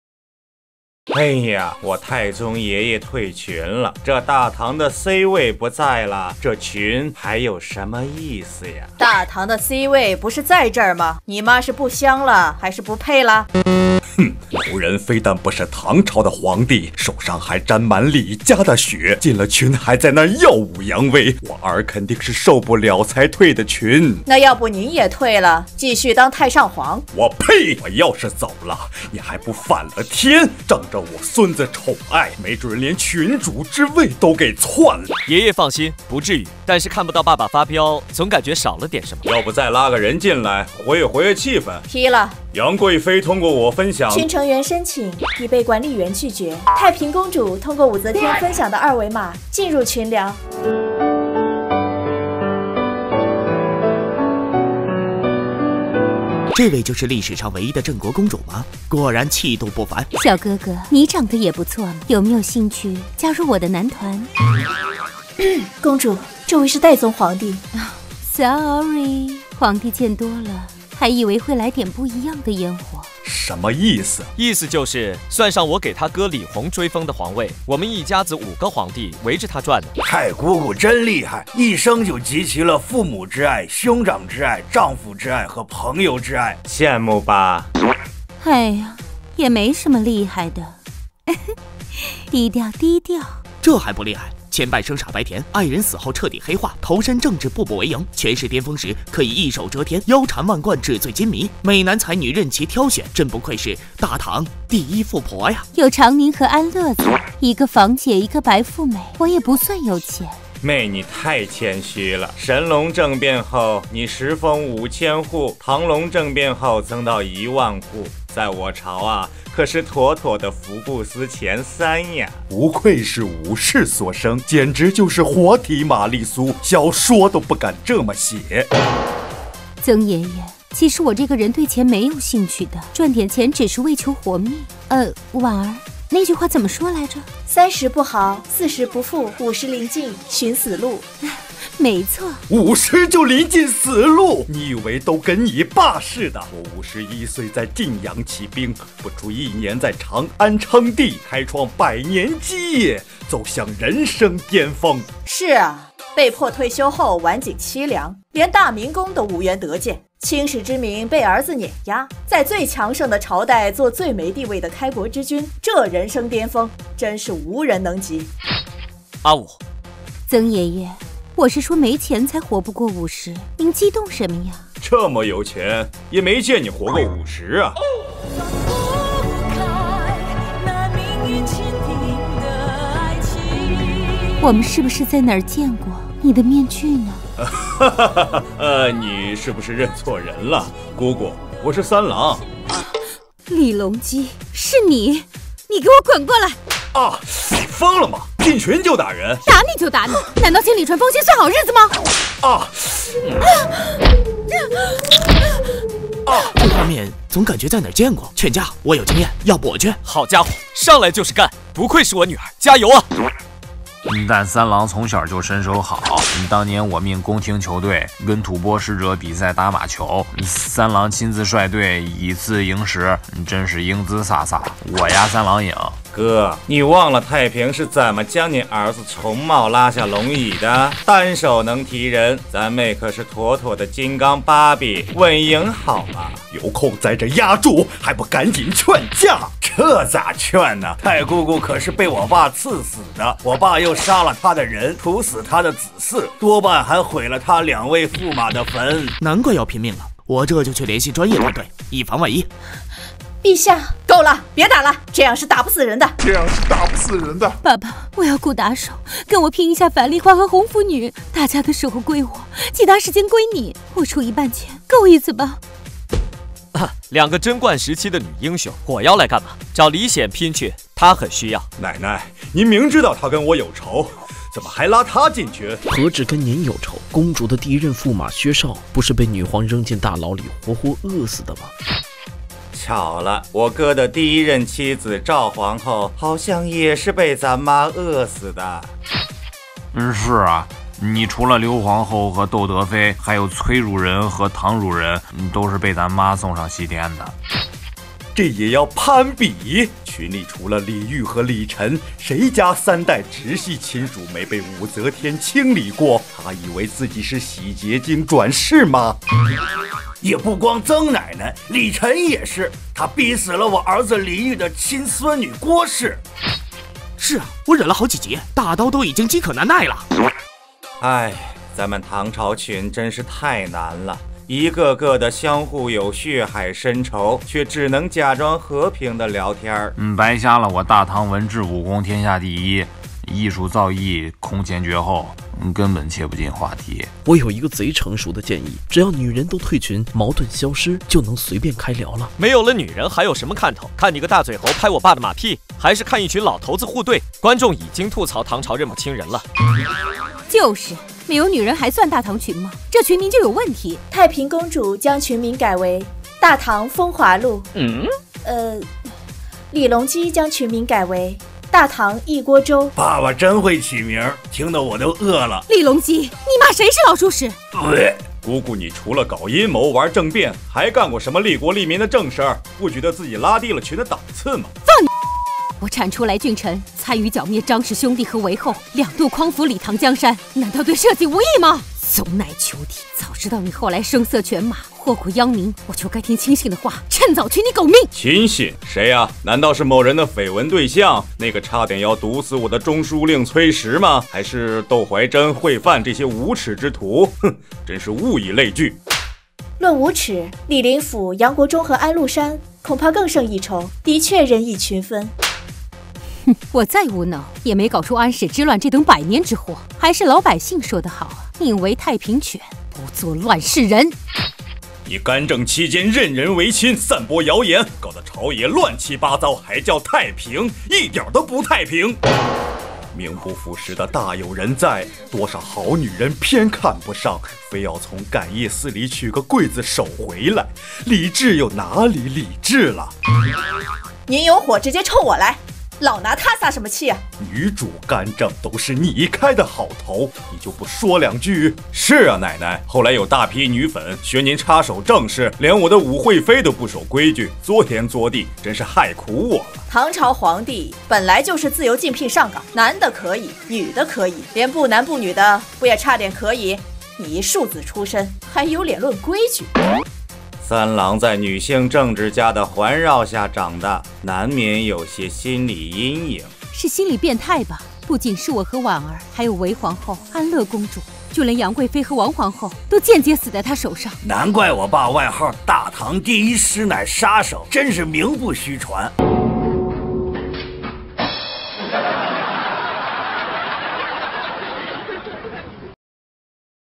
【胥渡吧】唐朝群聊（5）：大唐第一富婆太平公主进群
真名媛太平公主进群，开骂李隆基！